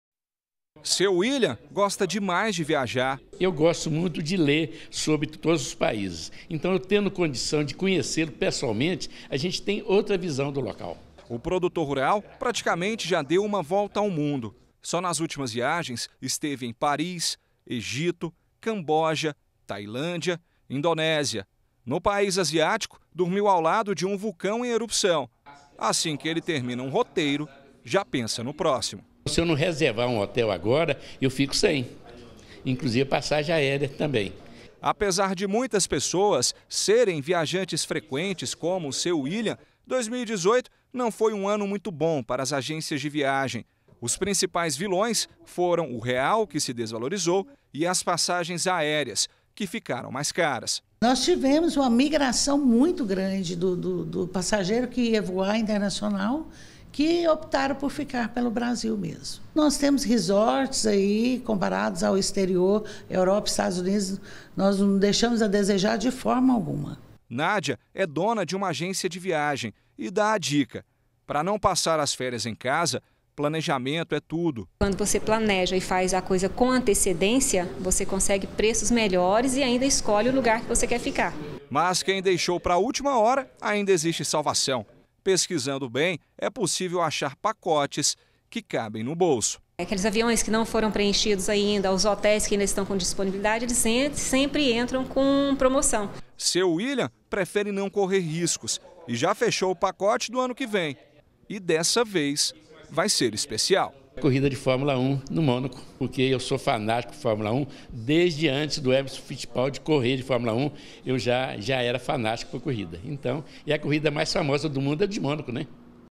VIAGEM DE FÉRIAS: PLANEJAMENTO AJUDA A REDUZIR GASTOS
O aumento do dólar durante o ano provocou uma alta de cerca de 20% nos pacotes de viagem e fez muita gente mudar de planos. Pra quem ainda vai fazer uma reserva pro fim de ano, uma dica é ficar atento às promoções.